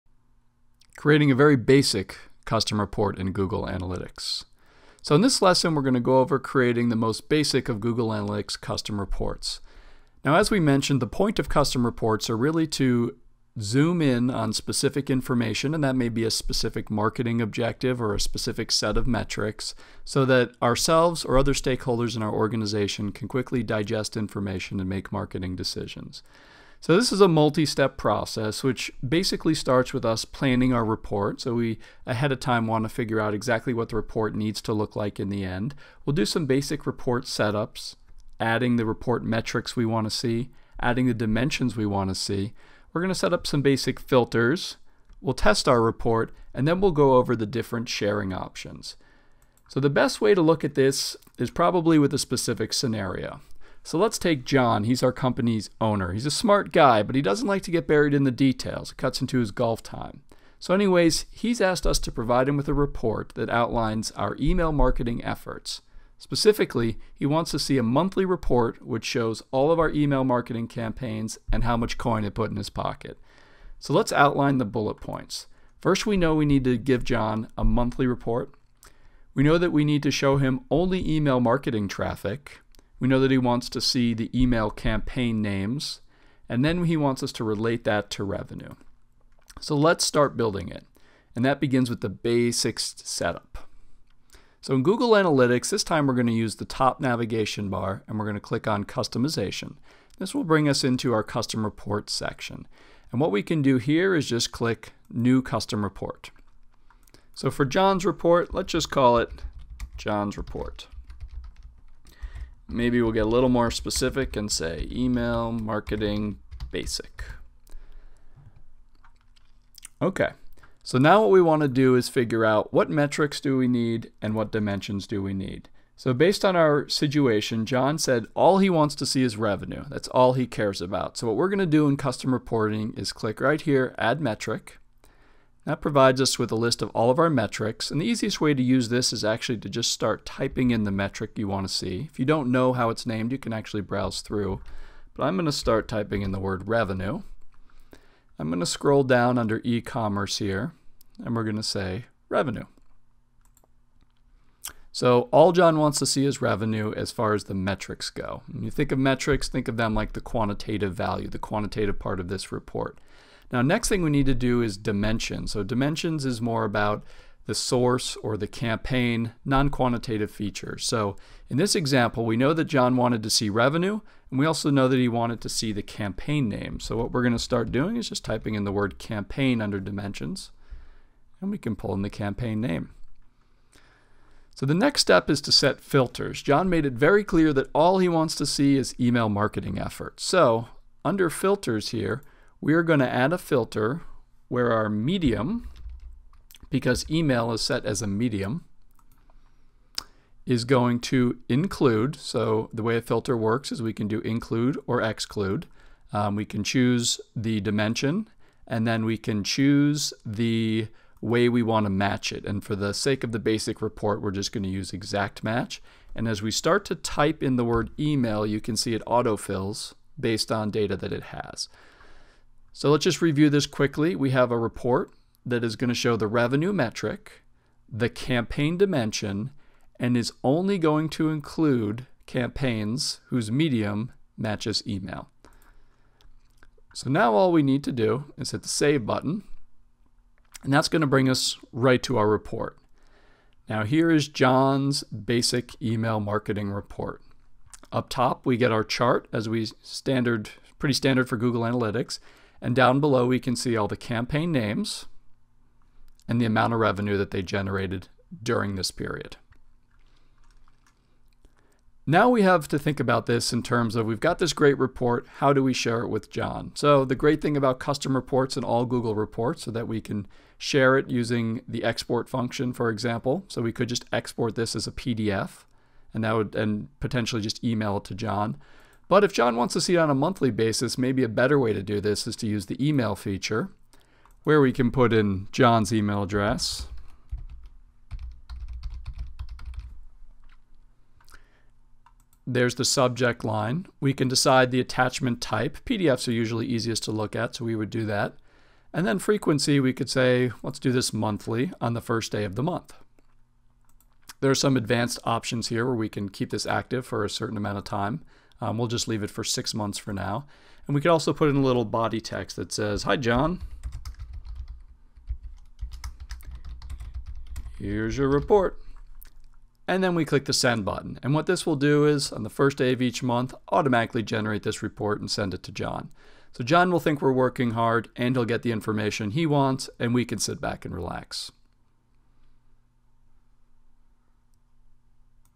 Basic Custom Report Creation in Google Analytics
Build custom reports within Google Analytics.